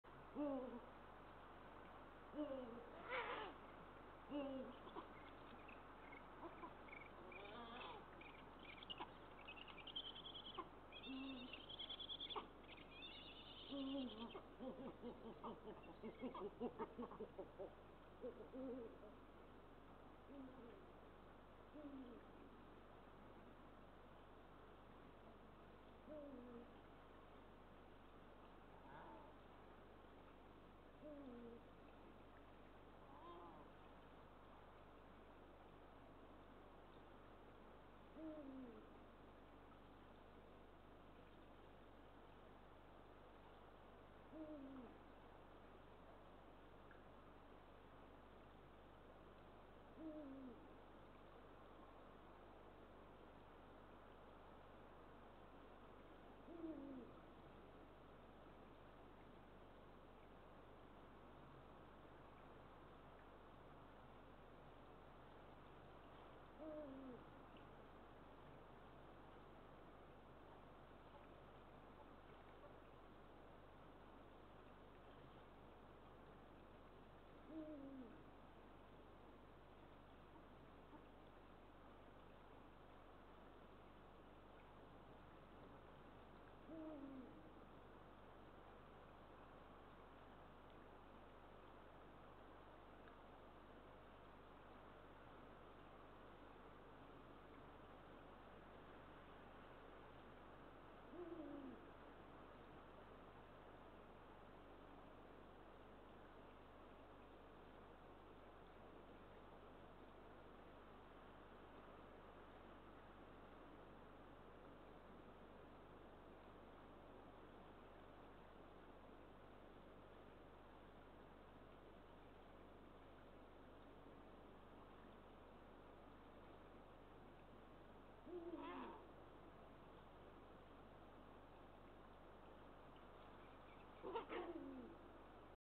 Hanna's dinner arrives. Estonian Eagle Owls, Kotkaklubi. 16 February 2024
Hugo brings Hanna a small rodent for dinner. :-)